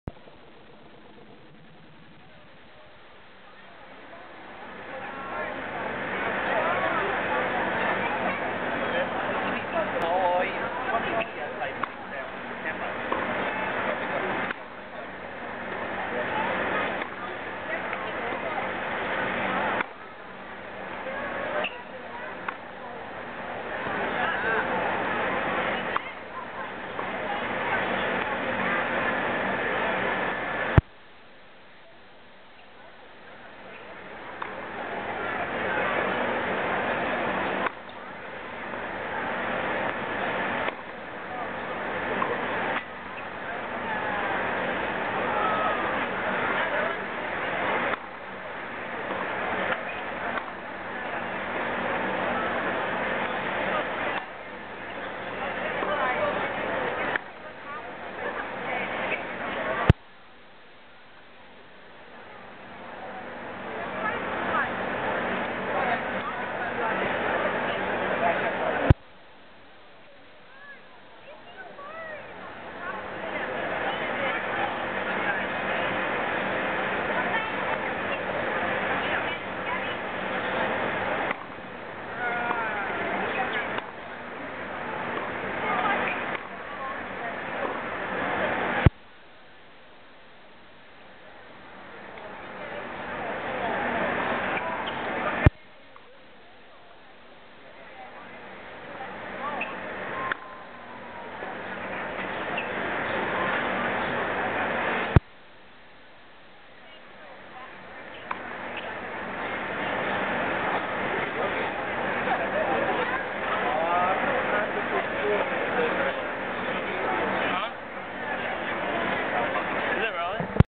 Vince Spadea Practices at 2009 Australian Open
Vince takes his last few groundstrokes before his first round match at the 2009 Australian Open.